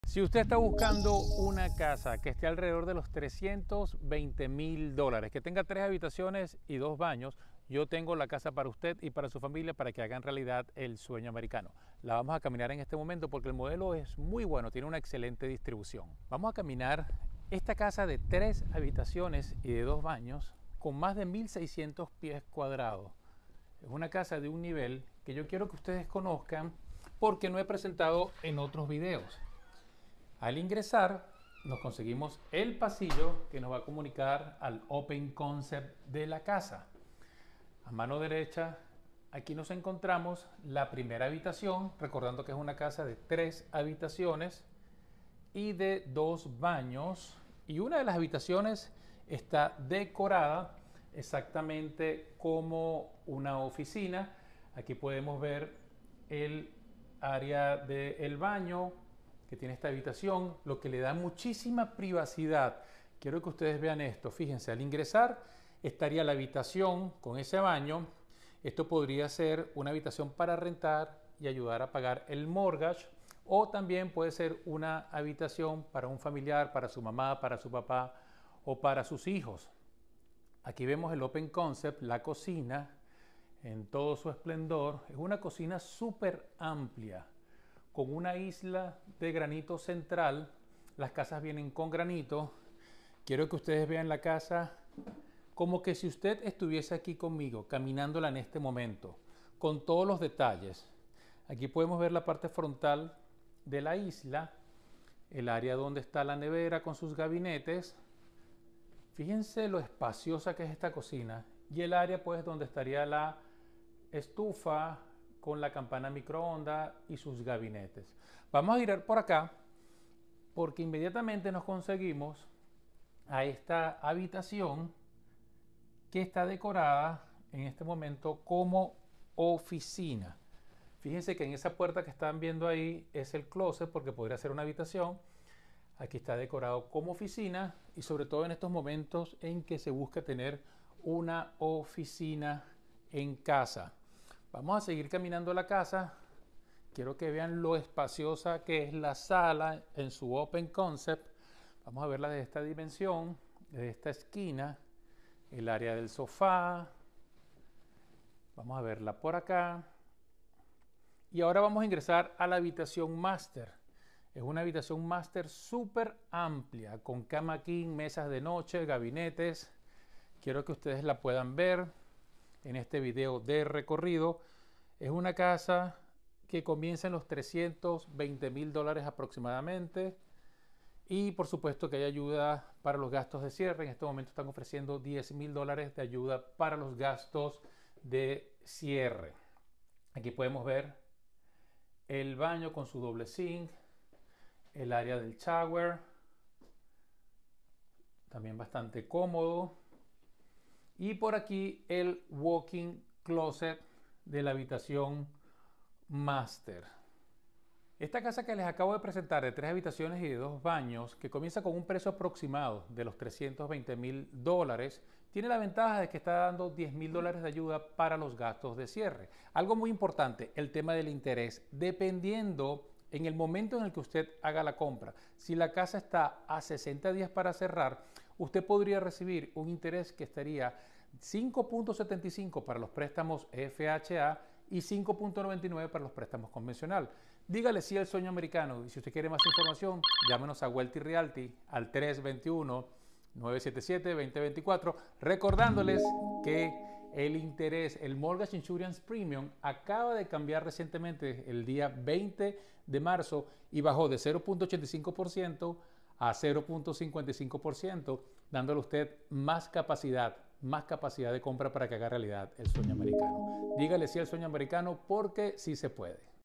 Casas NUEVAS en Davenport, Florida desde $320,000. Ayuda GASTOS CIERRE $10,000.
Casas Nuevas desde $320,000 en Davenport, Florida!!!

Comunidad espectacular con muchas amenidades: piscina, canchas, parques, entre muchas mas.

El builder ofrece ayuda de $10,000 para GASTOS de Cierre.
Esta casa es ideal para familias pequeñas o parejas que necesitan una oficina en la casa.

APROVECHA LA REDUCCION EN EL SEGURO HIPOTECARIO EFECTIVA DESDE EL 20 DE MARZO. AHORA TIENES MAS CAPACIDAD DE COMPRA!!!